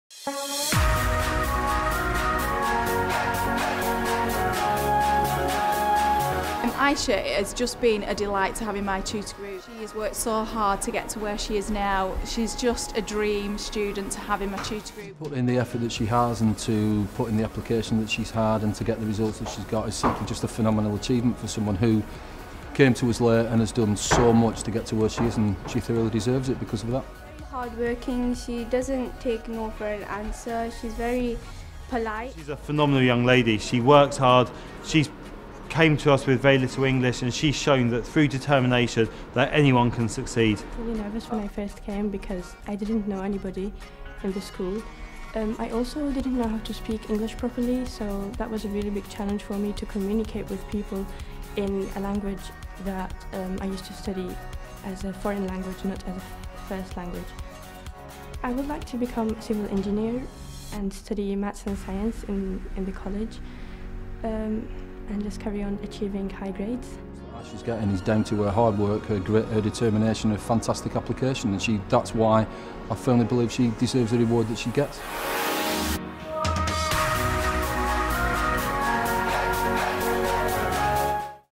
The Fusion Awards 2015 - Aysha Sultan UTC Bolton Education Achievement Award Winner
A short clip on the 2015 UTC Bolton Education Achievement Award Winner Aysha Sultan.